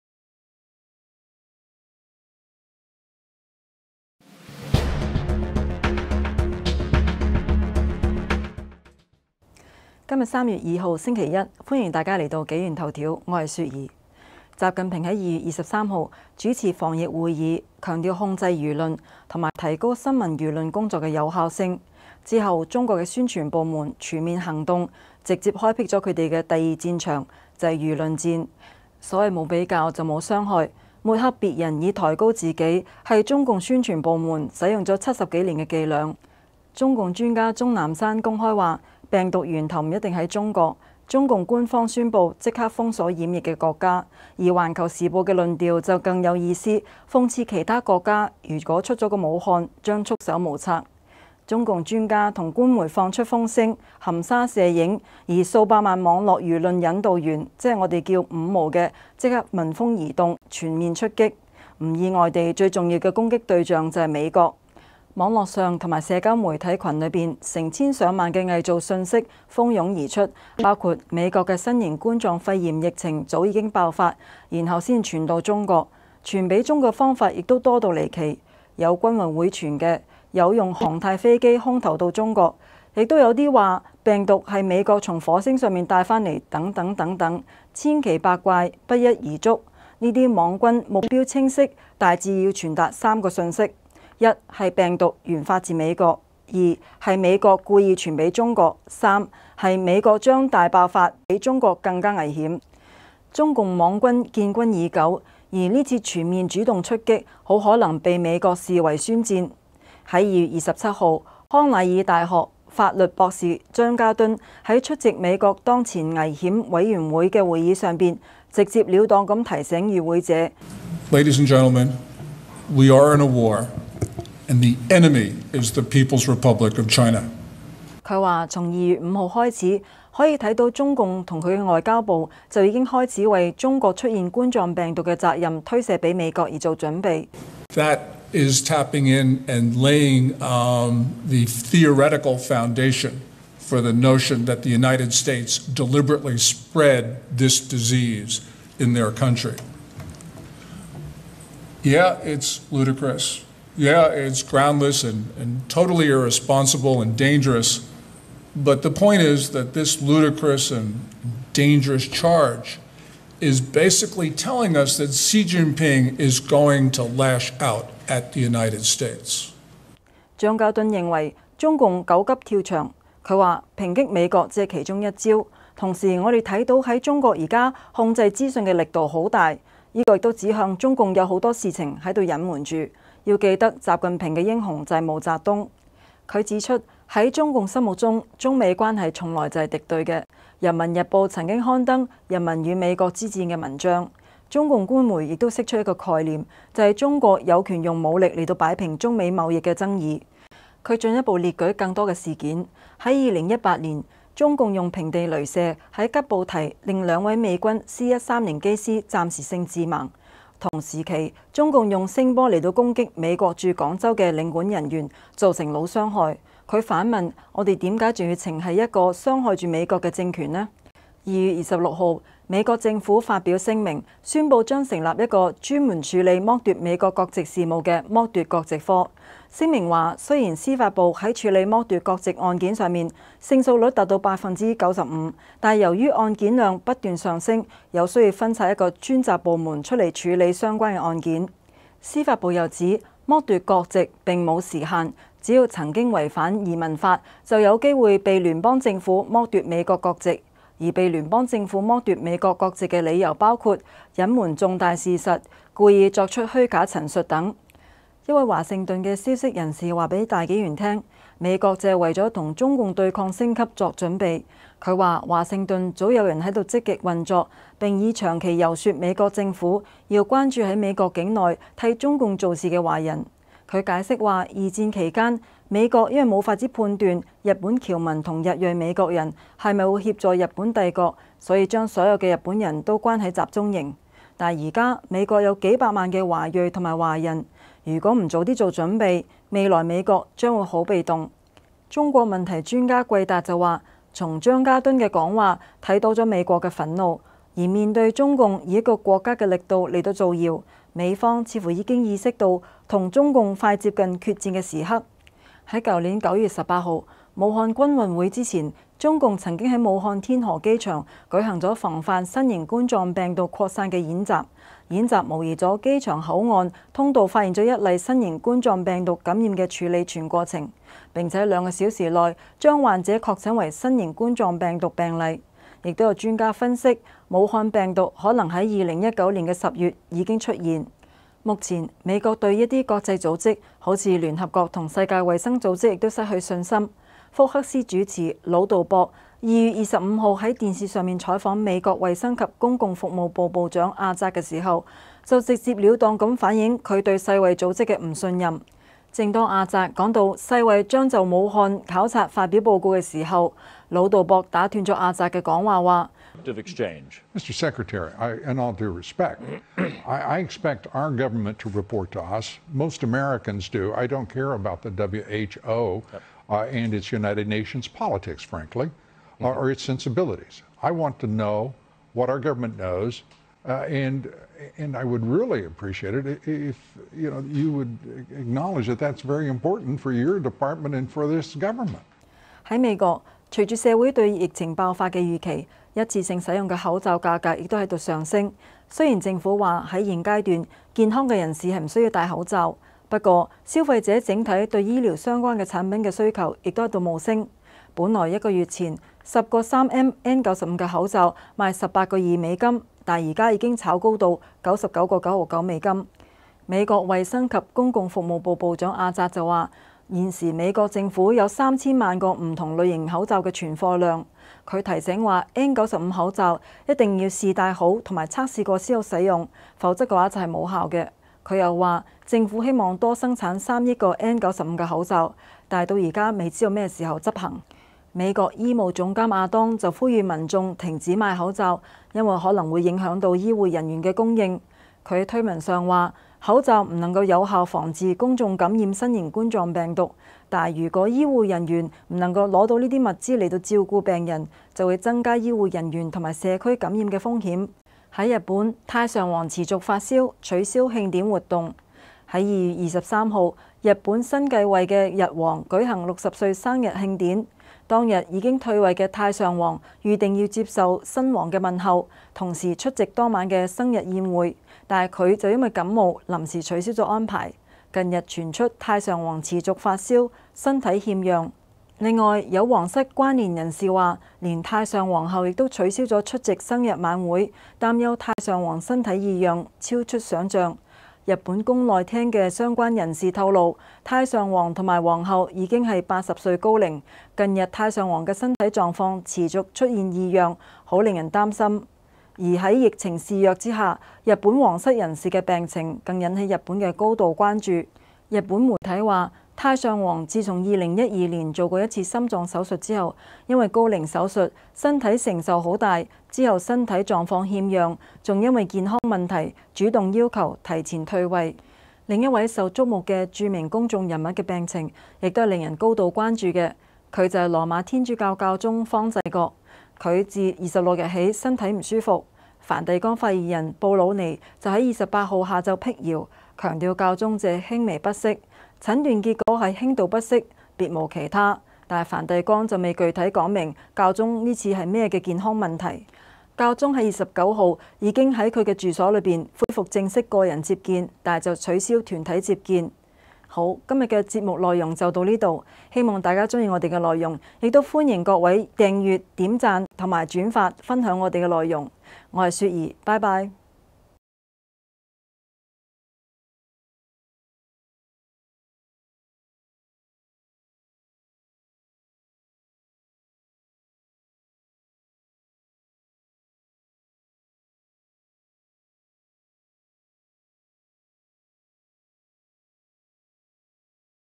【紀元頭條】中共大外宣對美開戰，美國準備應付對抗升級，成立專門機構，日本太上王感冒發燒，天主教教宗身體不適，惹外界高度關注。
習近平在2月23日主持防疫會議強調控制輿論，和「提高新聞輿論工作的有效性」。之後中國的宣傳部門全面行動，直接開闢了他們的第二戰場，也就是輿論戰。

所謂沒有比較就沒有傷害。抹黑別人，以抬高自己，是中共宣傳部門使用了七十多年的伎倆。

中共專家鐘南山公開表示，病毒源頭「不一定是中國」，中共官方宣布立即封鎖染疫的國家，而《環球時報》的論調更有意思，嘲諷其他國家，如果出了個武漢，將束手無策，。
中共專家和官媒放出風聲，含沙射影，而數百萬網絡輿論引導員，也就是一般認為的五毛，則聞風而動，全面出擊。
不意外地，最重要的攻擊對象是美國。

網絡上和社媒群中，成千上萬的偽造信息蜂擁而出，包括美國的新冠肺炎疫情早已爆發，然後才傳到中國，傳給中國的方法也多到離奇，有軍運會傳的，有用航太飛機空投到中國，還有的說這種病毒是美國從火星上帶回來的，等等等等，千奇百怪不一而足。這些網軍目標清晰，大致要傳達三個信息：

一是病毒原發自美國；
二是美國故意傳給中國，
三是美國將大爆發，比中國更為危險。

中國網軍建軍已久，而這次全面主動出擊，很可能被美國視為「宣戰」。

2月27日，康乃爾大學法律博士章家敦，在出席美國當前危險委員會（Committee on the Present Danger: China）的會議上，直接了當提醒與會者說：「各位，我們已經在打仗，而我們的敵人就是中國（共）。」

他表示，從2月5日開始，我們看到中共和其外交部，就已經開始將中國出現冠狀病毒的責任推卸給美國做準備，「這是為造謠，指美國有意在中國釋放這個病毒的說法打下理論上的基礎。這是荒謬的、完全沒有跟據，不負責任而危險的，重點是這種荒謬和危險的指控在告訴我們，習近平在抨擊美國。」

章家敦認為，中共狗急跳牆，「抨擊美國只是其中一招，同時我們看到在中國現在控制資訊的力度很大，這也是指向中共知道有很多事情是要隱瞞的。要記得習的英雄是毛澤東. 」

他指出，在中共心目中，美中關係從來就是敵對的，人民日報曾刊登「人民與美國之戰」的文章，中共官媒也釋出一個概念：「中國（共）有權用武力來擺平中美貿易爭議」。

他進一步列舉更多的事件：2018年，中共用平地雷射，在吉布提（Djibouti）令2位美軍C130機師暫時性致盲，同時期，中共用音波來攻擊美國駐廣州的領館人員，造成腦傷害，我們不禁問，我們為何還要情繫一個在傷害著美國的政權？！

2月26日，美國政府發表聲明，宣佈將成立一個專責處理剝奪美國國籍事務的「剝奪國籍科 (Denaturalization Section)」。聲明指，雖然司法部在處理剝奪國籍案件上，勝訴率達95%，但由於案件量不斷上升，有需要分拆一個專責部門出來處理相關案件。

司法部又指，剝奪國籍並沒有時限 (no statute of limitations)，只要曾經違反移民法，就有機會被聯邦政府剝奪美國國籍，而被聯邦政府剝奪美國國籍的理由包括，隱瞞重大事實 (concealment of a material fact)，故意作出虛假陳述 (willful misrepresentation) 等。

一位華盛頓的消息人士告訴大紀元，美國正在為與中共對抗升級作準備。他說，華盛頓早有人在積極運作，並已長期遊說美國政府，要關注在美國境內的替中共做事的華人。他解釋說，二戰期間，美國因無法判斷日本僑民和日裔美國人是否會協助日本帝國，因此把所有日人關入集中營。但現在，美國有幾百萬的華裔和華人，如果不早做準備，未來美國將非常被動。

中國問題專家季達說，從章家敦的講話，看到了美國的憤怒，在面對一個中共以一個國家的力度來造謠，美方似乎已經意識到與中共快接近決戰的時刻。

去年2019 年9月18日，武漢軍運會前，中共曾在武漢天河機場舉行了防範「新型冠狀病毒擴散」演習。演習模擬了機場口岸通道發現1例“新型冠狀病毒”感染的處置全過程，並在2小時後將「患者」確診為「新型冠狀病毒」病例。

也有專家分析，武漢病毒可能在 2019 年 10 月就已經出現

目前，美國對一些國際組織，如聯合國和世界衛生組織等也失去了信任。福克斯主持人魯道博（Luo Dobb）於2月25日在電視上採訪美國衛生及公共服務部部長阿札（Alex Azar）時，就直接了當的反映他對世衛組織的不信任。

正當阿札談到世衛將就武漢考察發表報告時，魯道博打斷了阿札的講話說，「我期望的是我們自己政府向我們匯報，老實說，我們不在乎世衛組織以及聯合國完的任何『政治』，我需要知道我們政府掌握的情況。」

在美國，隨著社會對疫情爆發的預期，一次性使用的口罩價格也在上升，雖然政府說在現階段健康的人士不需要戴口罩，不過，消費者整體對醫療相關的產品的需求在冒升。本來10個3M N95口罩賣18.20美元，現在已經被炒高到99.99美元。

美國衛生及公共服務部部長阿札說，現時美國政府有3千萬不同類型口罩的存貨量，他提醒說，N95口罩一定要試戴好和測試過才使用，否則是無效的。他又說，政府希望多生產3億個N95口罩，但現在仍不知道什麼時候執行。

美國醫務總監阿當（Jerome M. Adams ），則呼籲民眾停止買口罩，因為可能會影響到醫護人員的供應。他在推文上說，口罩不能有效防治公眾感染新冠狀病毒，但如果醫護人員沒能拿到這些物資來照顧病人，將會增加醫護人員和社區感染的風險。

日本太上王持續發燒  取消慶典活動

2月23日，日本新繼位日王舉行60歲生日慶典。當日，已退位的太上王，預定接受新王的問候，同時出席當晚的生日宴會，但他卻因感冒，臨時取消了安排。近日，傳出太上王持續發燒，身體欠恙。

另有王室關連人士表示，連太上王后也取消了出席生日晚會，擔憂太上王身體異樣超出想像。

日本宮內廳的相關人士透露，「太上王及王后已是80高齡，近日太上王的身體狀況持續出現異樣，很讓人擔憂。」

在疫情肆虐之下，日本皇室人士的病情，引起日本的高度關注。

日本媒體水，太上王自從2012年做過一次心臟手術後，因高齡手術，身體承受很大，之後身體狀況欠恙，還因健康問題，主動要求提前退位。

另一位受觸目的著名公眾人物的病情，也是令人高度關注；羅馬天主教教宗方濟各（Pope Francis）自26日起身體不適，梵蒂岡發言人布魯尼（Matteo Bruni）在28日下午闢謠，強調教宗只是輕微不適，「診斷結果是輕度不適，別無其他」。但梵蒂岡未具體說明教宗此次健康問題為何。

教宗在29日已在其住所恢復正式個人接見，但取消團體接見。

責任編輯：Lisa/Sylar